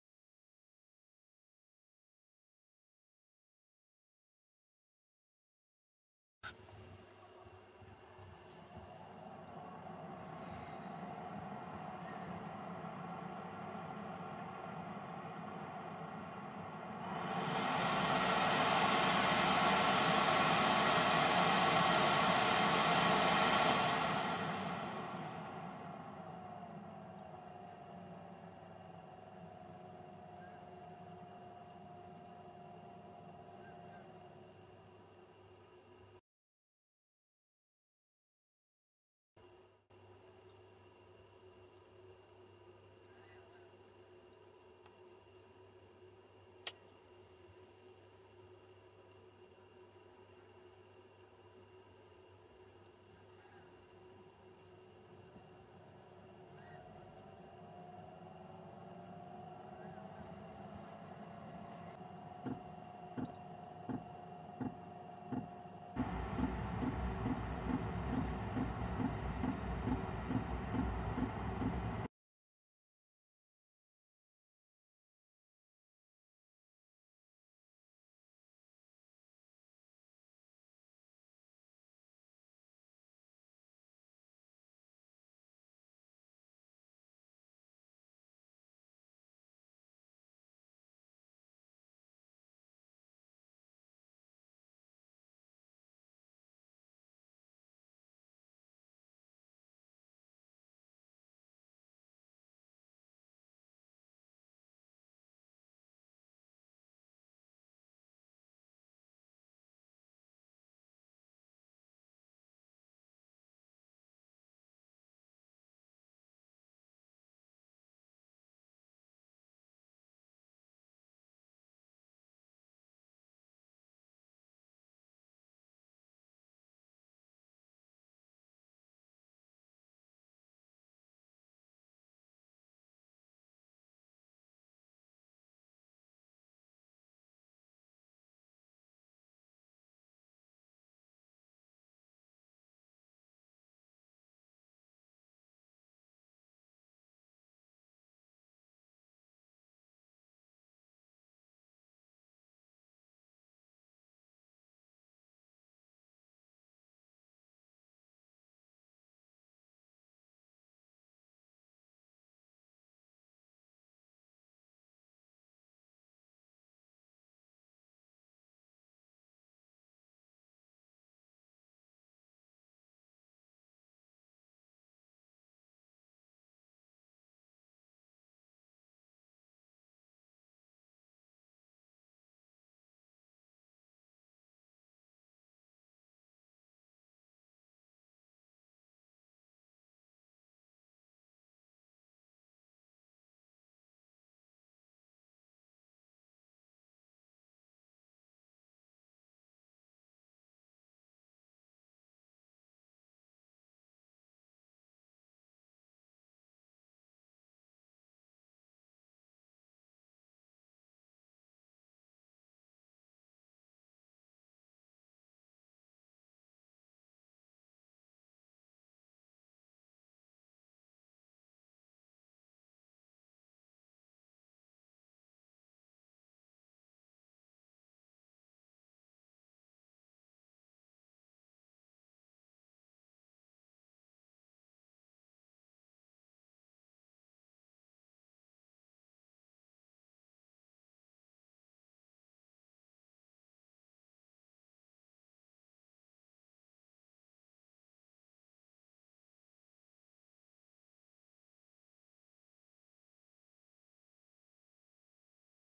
Vanlife China Diesel Heater Smartphone remote (Bluetooth) + FAN control for ventilation
simple to connect via smartphone and easy to use. works plug and play

the system uses less energy than the original display.

also the feature to just turn on the fan was not possible with the original control. perfect for van's or camping.